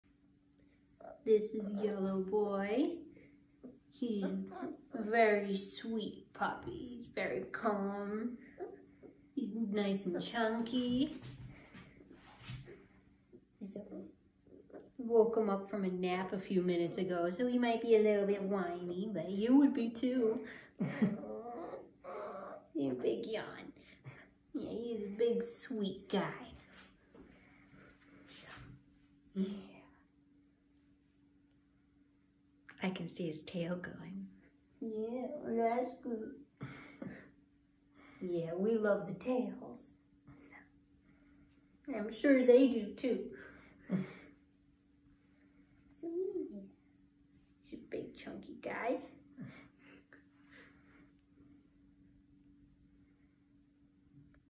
Holding Ellie’s Yellow Boy Old English Sheepdog Puppy
This is an Old English Sheepdog puppy born March 15, 2022, to Ellie x Wally.

Wisconsinoldenglishsheepdogs.com